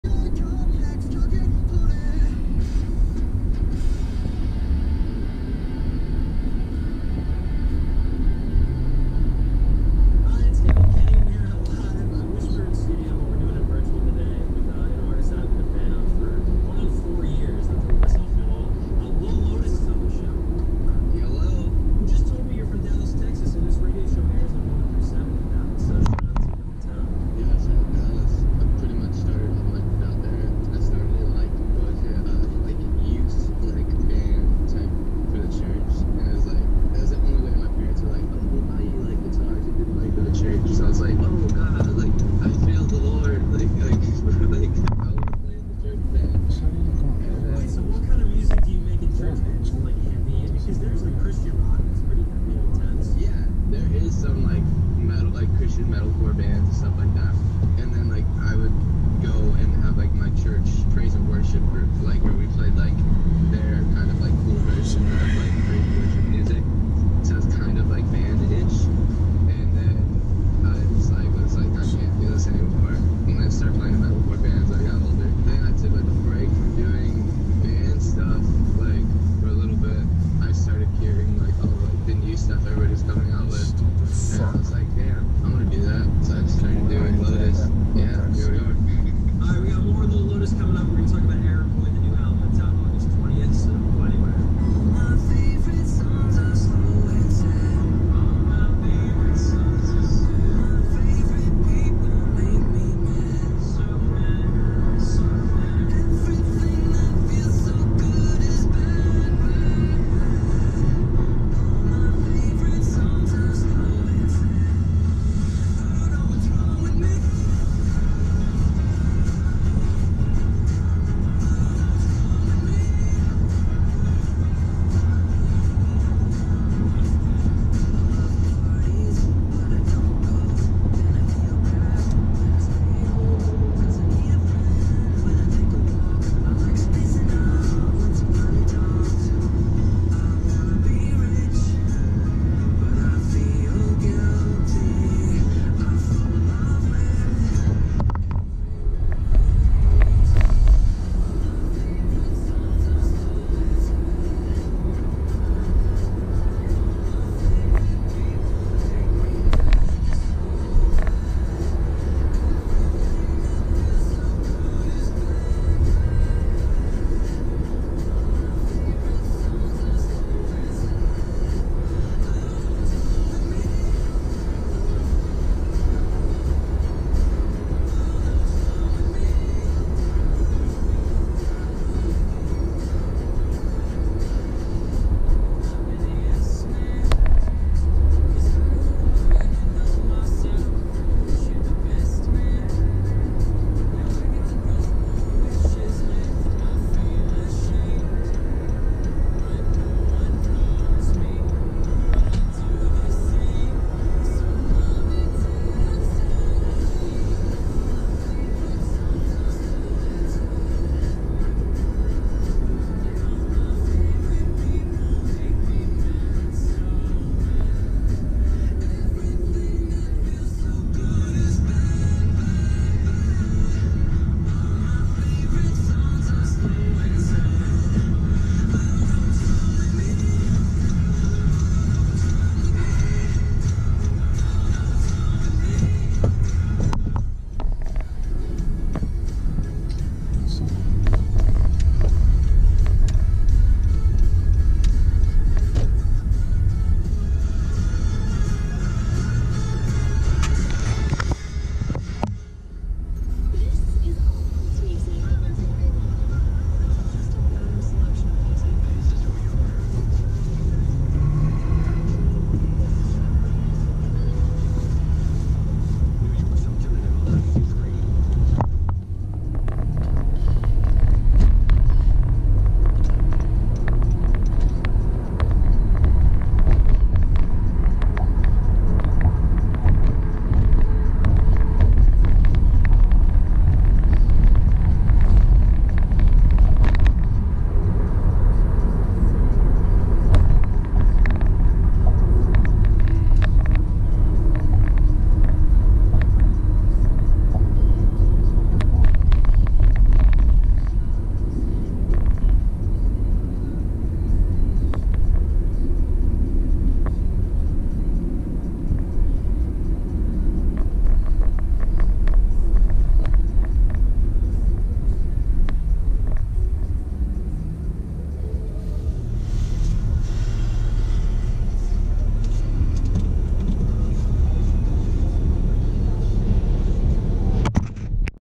Driving from Bronx to uptown Manhattan
Driving on the Grand Central PK New York City. RFK Bridge to Bronx and uptown Manhattan. Sunset, evening afternoon view